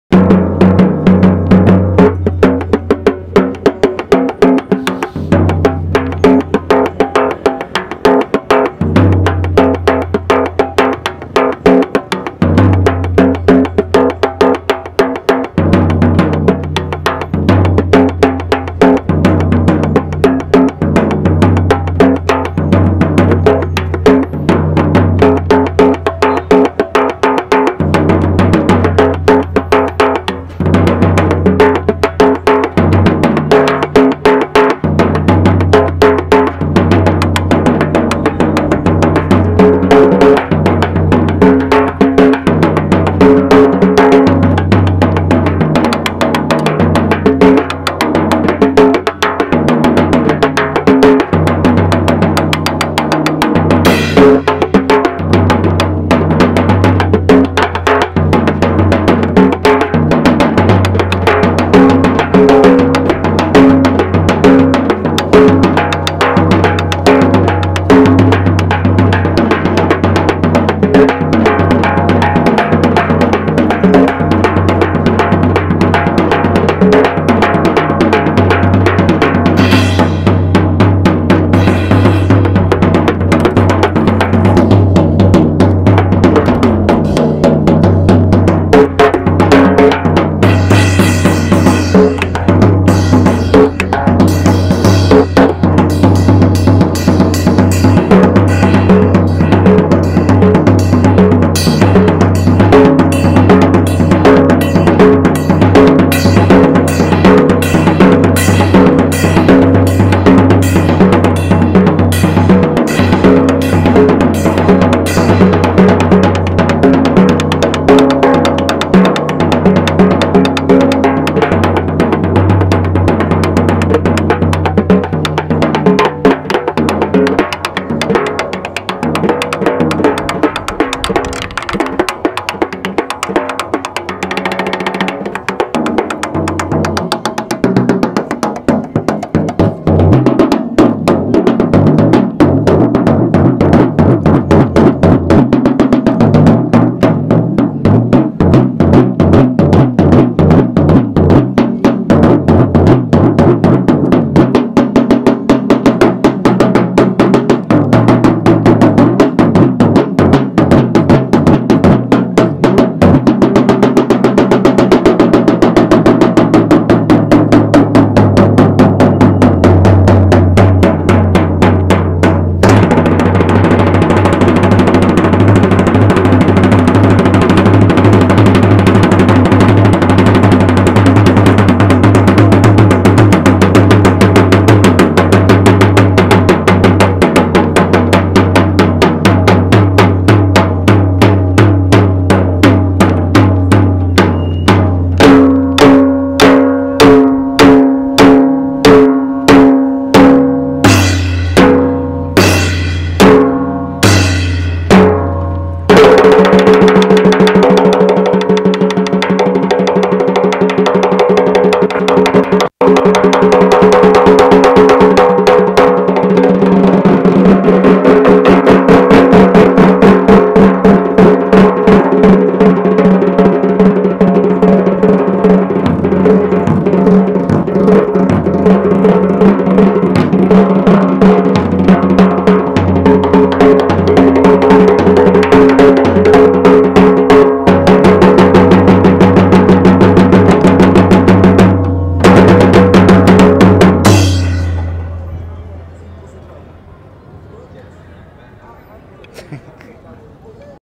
Danza y percusiones en plaza libertad. San Salvador. El Salvador.
Comparto con ustedes un performance realizado por Georgina (bailarina nicaragüense) y Fernando Barrios (percusionista salvadoreño).